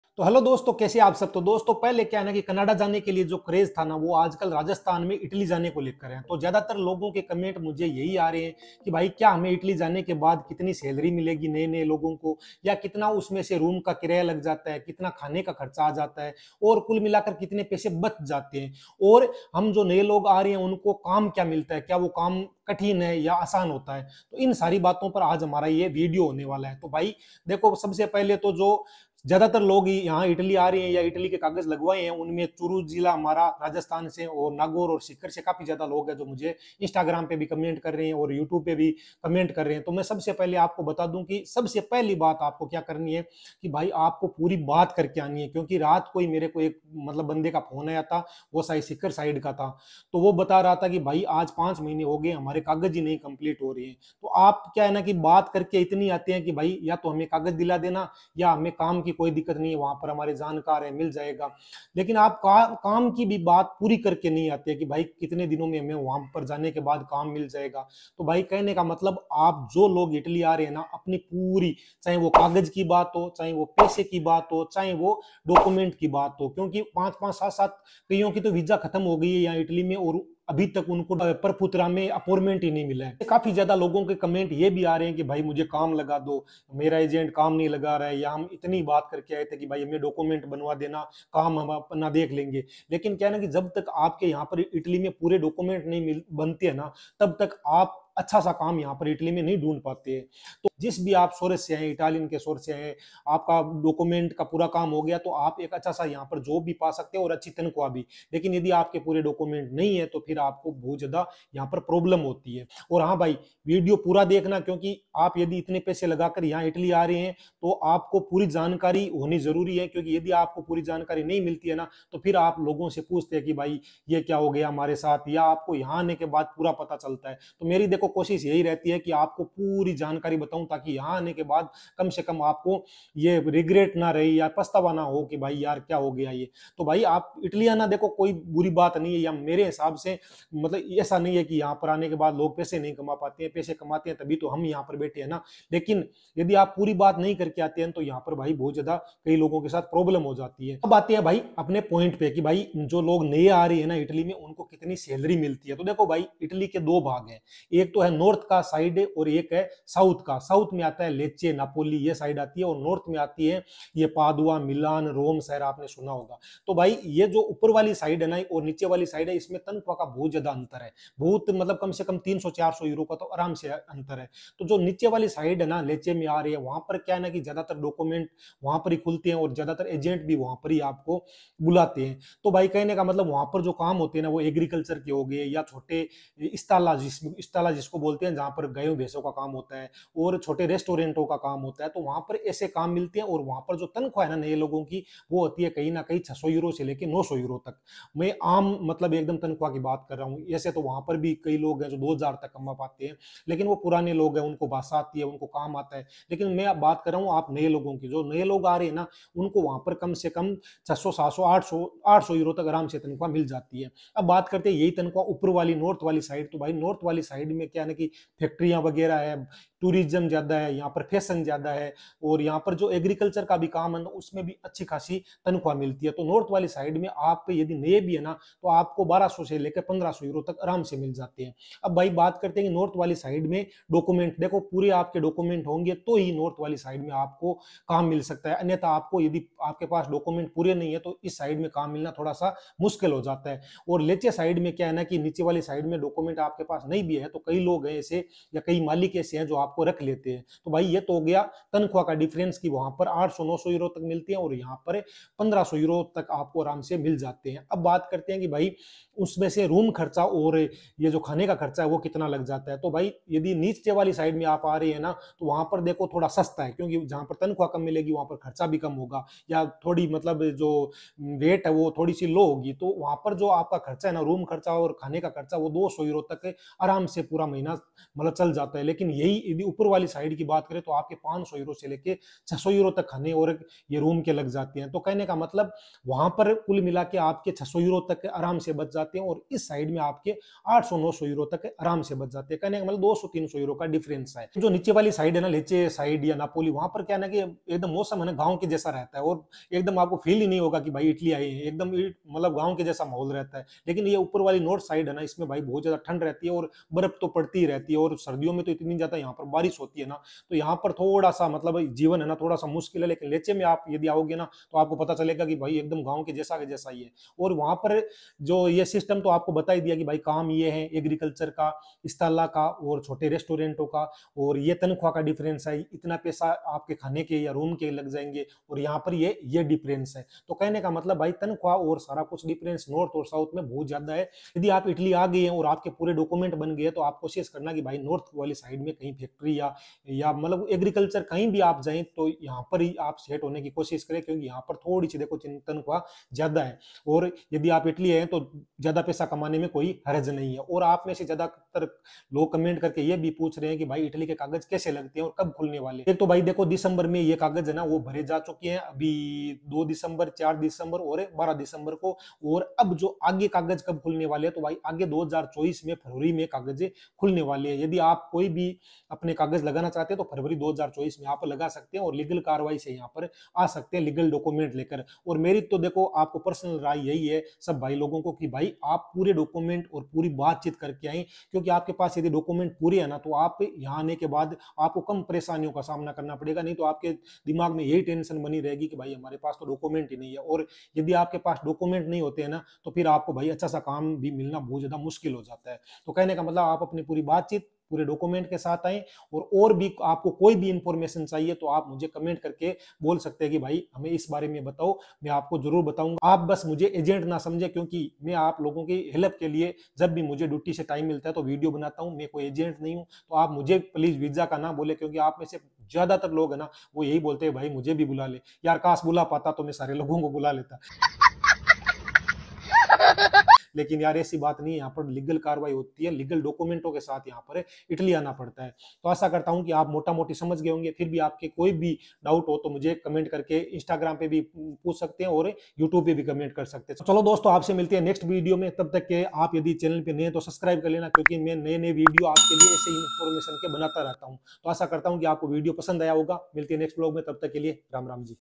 Italy Salary information / इटली मैं सैलरी और रहने का ख़र्चा कितना है / Avi Italian vlogs
हेलो दोस्तों इस वीडियो में इटली की सैलरी प्लस रूम किरायाऔर बचत के बारे में बताया गया कि इटली में कितनी सैलरी मिलती है और कितनी बचत होती है आशा करता हूं कि आपको वीडियो पसंद आएगा वीडियो पसंद आया हो तो चैनल को सब्सक्राइब और वीडियो को लाइक और शेयर जरूर करना आपका भाई अविनाश